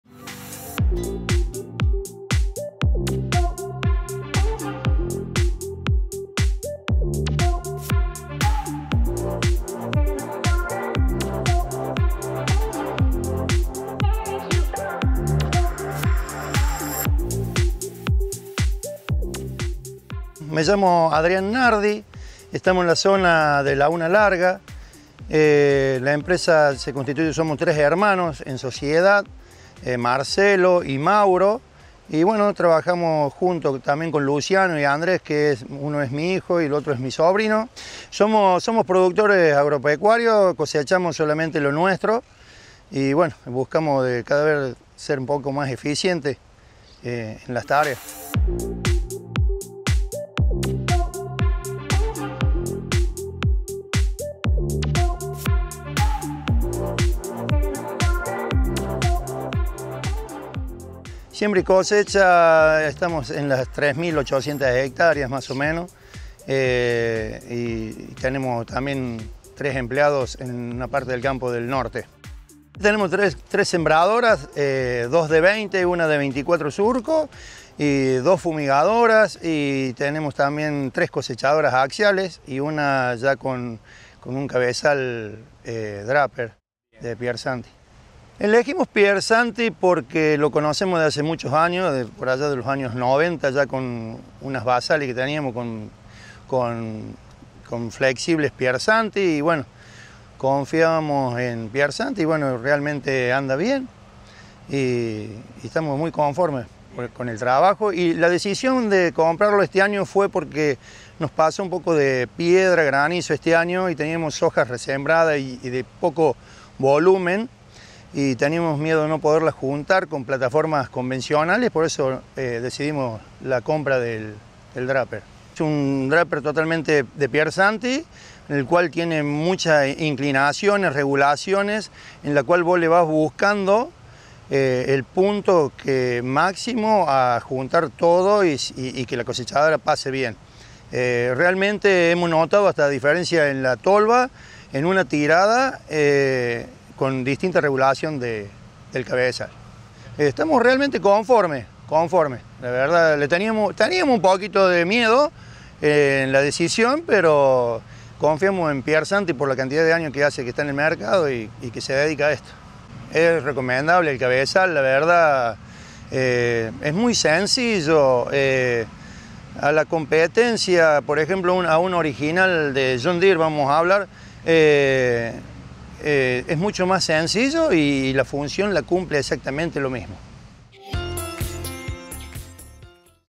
Probamos el nuevo draper Piersanti en Laguna Larga
Junto a la familia Nardi cosechamos soja en Laguna Larga. Probamos el nuevo draper de Piersanti.